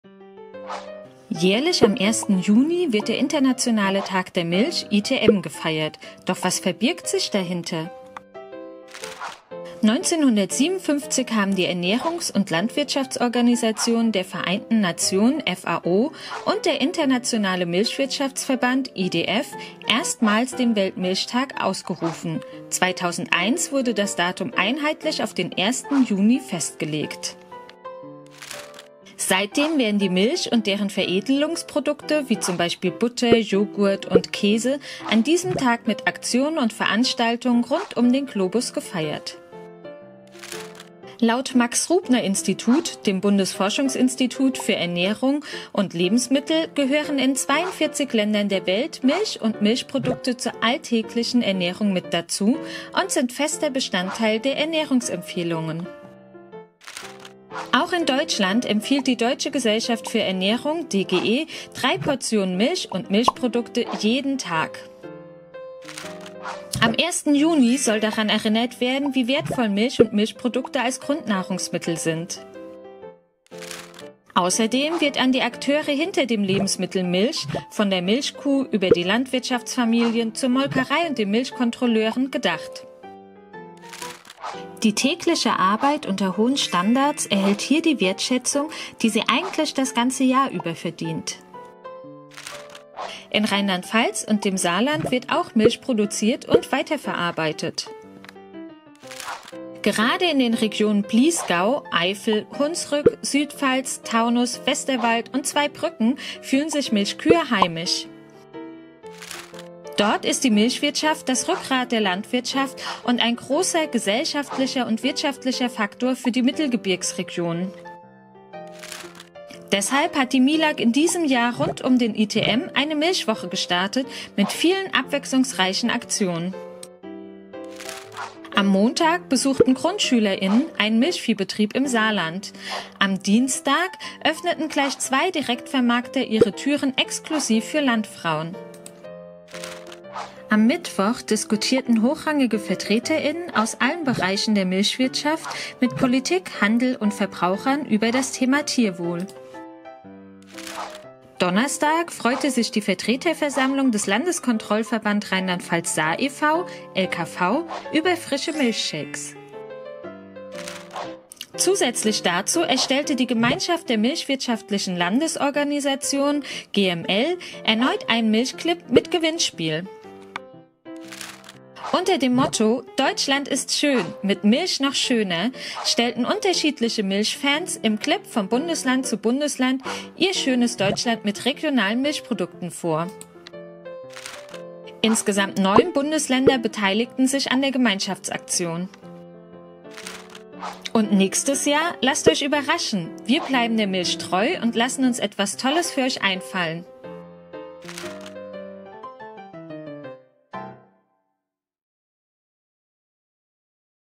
Wissenswertes rund um den Weltmilchtag
Woher kommt der Tag? Welche Bedeutung hat er? Und welche Aktionen waren in Rheinland-Pfalz und dem Saarland an diesem Tag?